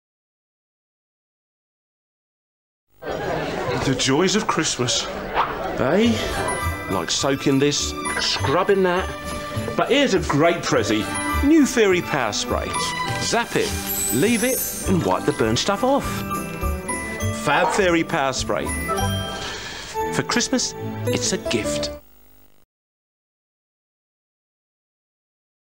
2003: Fairy Power Spray [Joys Of Christmas]
Title: Joys Of Christmas
Brand: Fairy
Advertiser/Client: Procter & Gamble
Agency: -
Media: TV Advert
Directed By: -
Featuring: Ainsley Harriet (British Chef)
Guest Appearance: -
Voice Over: -
Year: 2003
Location: -
Country: -
Dishwasher: -
Model: -
Slogan: For Christmas It’s A Gift.